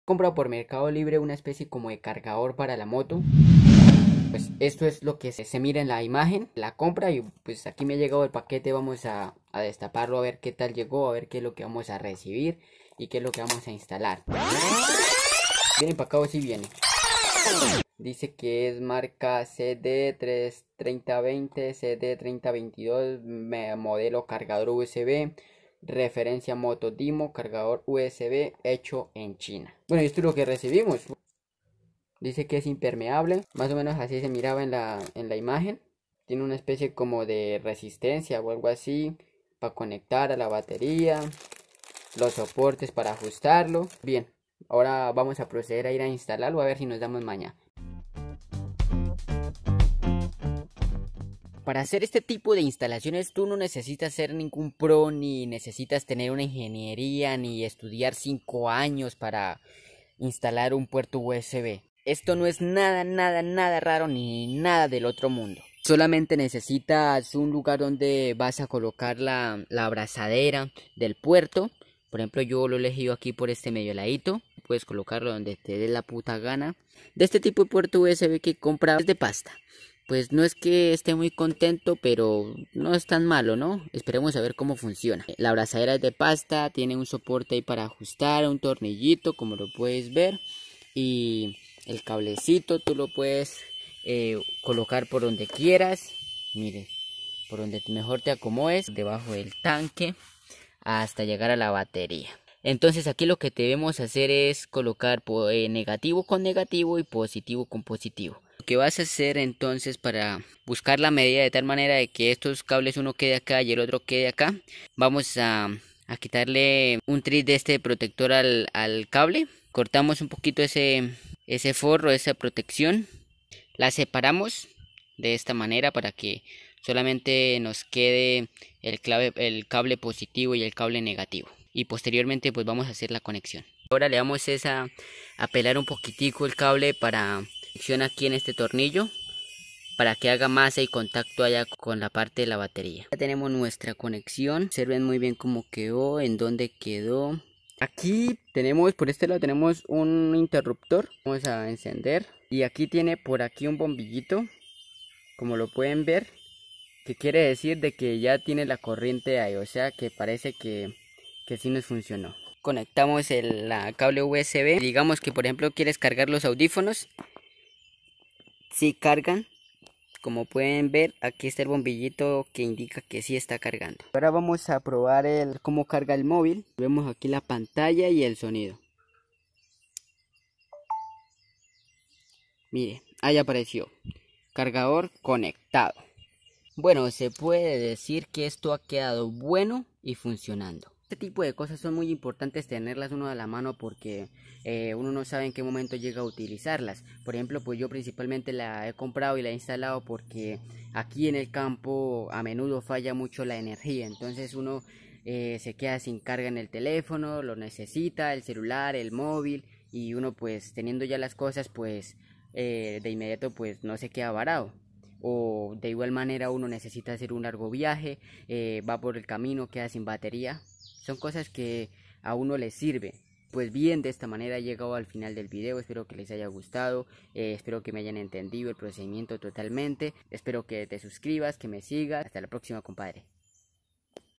Como instalar CARGADOR de CELULAR para MOTO doble puerto IMPERMEABLE
Tutorial como instalar puerto usb para cargar movil en tu moto.